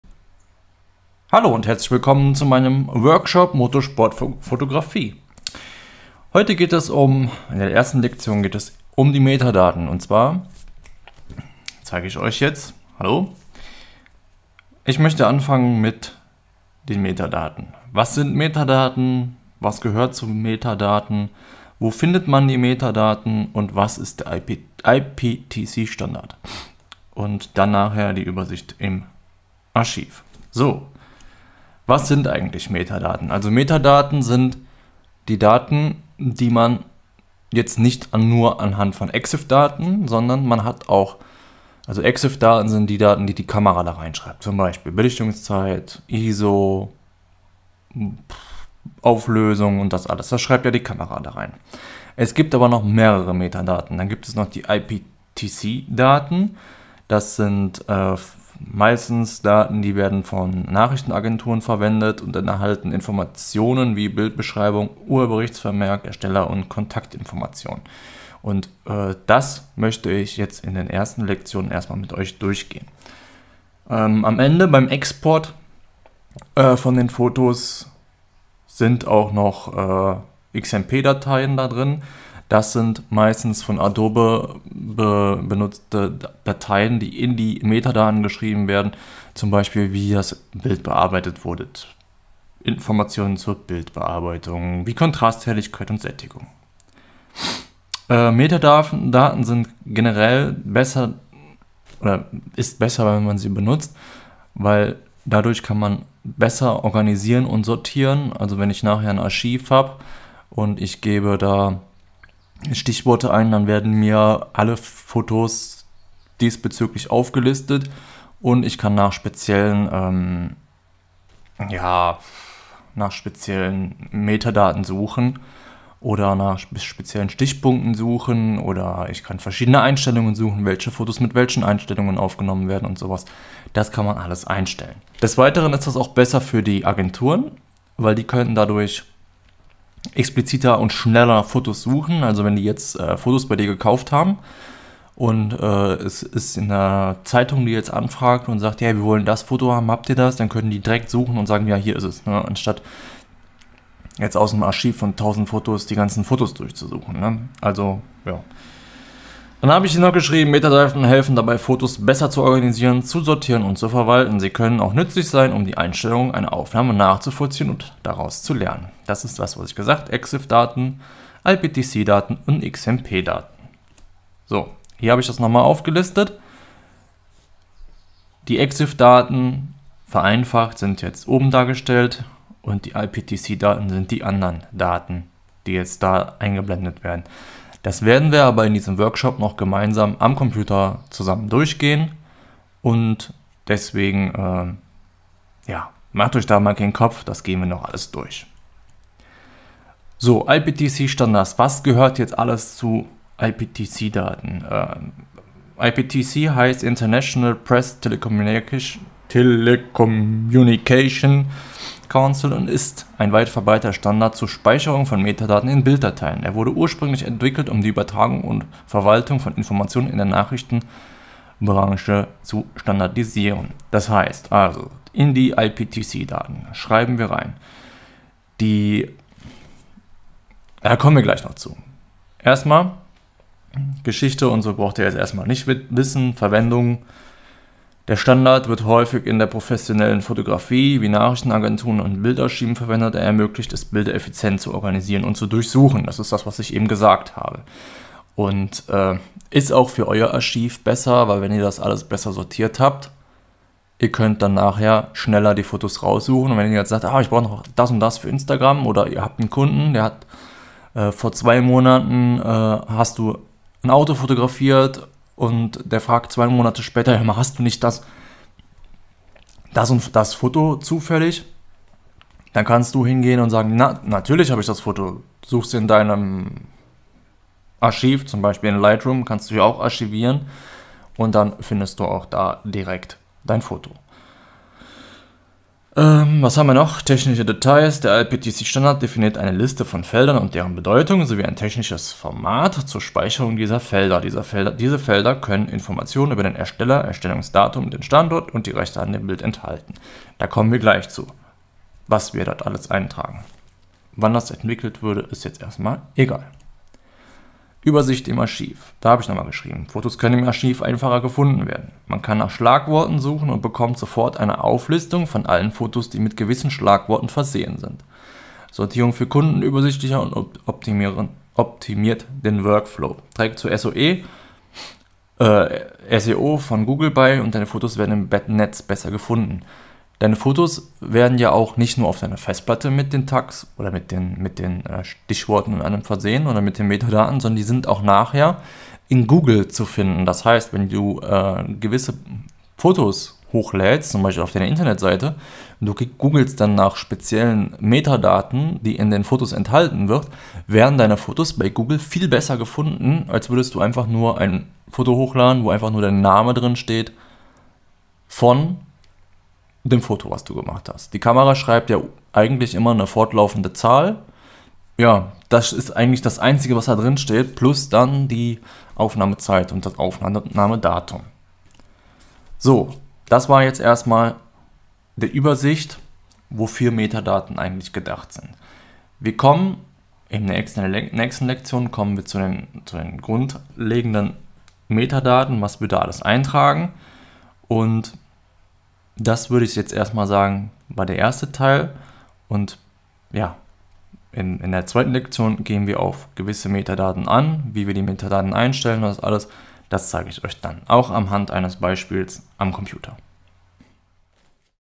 Motorsportfotografie Workshop Lektion #1: Metadaten Grundlagen
Motorsportfotografie - Pressefotografie - Metadaten - Adobe Bridge

In diesem Workshop lernst du, was Metadaten sind, für wen sich wichtig sind und wie du diese erstellst und bearbeitest. Weiterhin lernst du wie du Adobe Bridge bedienen kannst und wie du deinen Workflow an der Rennstrecke optimieren kannst.